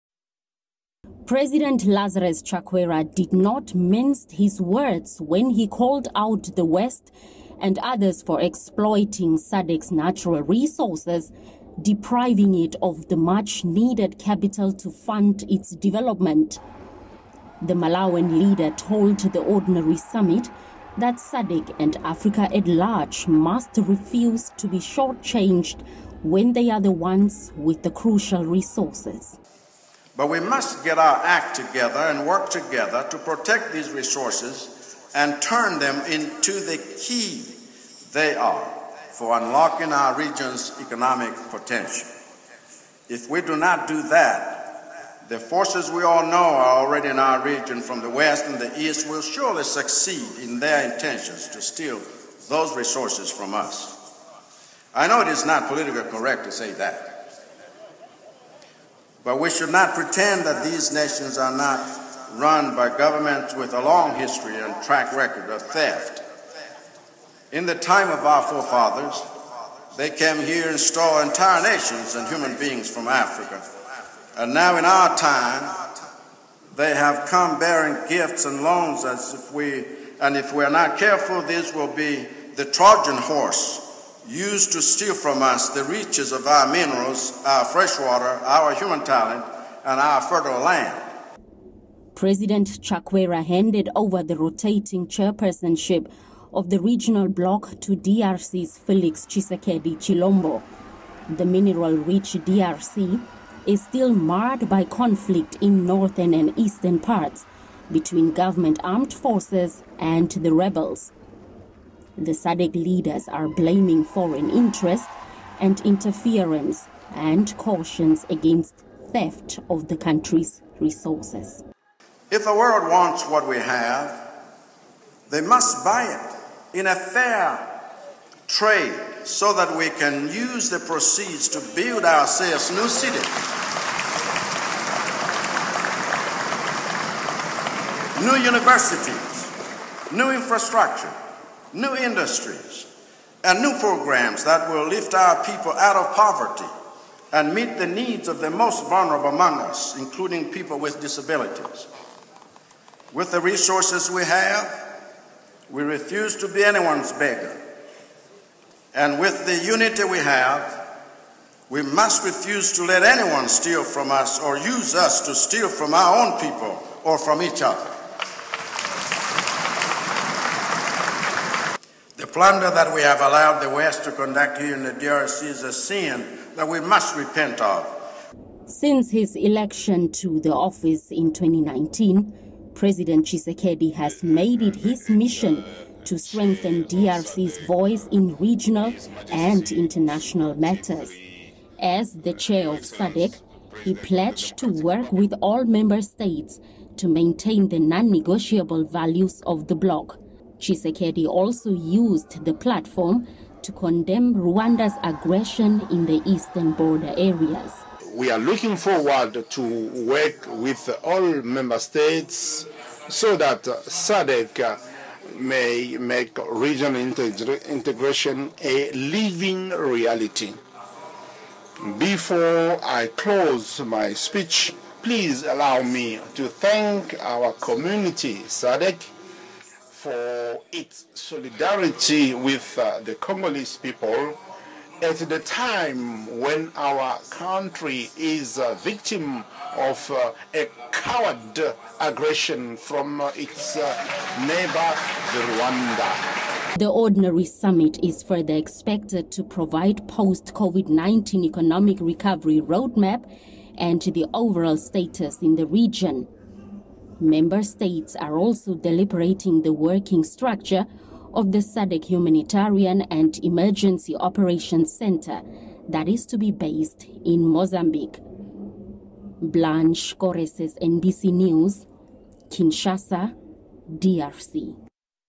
Outgoing SADC Chairperson wants member countries guard against foreign interference - nbc
The outgoing chairperson of SADC and Malawian President Lazarus Chakwera wants the region to guard against foreign interference and theft of the region's natural resources.

Chakwera says it has become crucial for the region to use the abundance of resources and fund projects for socio-economic development.

SADC Heads and Government are meeting in Kinshasa, Democratic Republic of Congo for the 42nd ordinary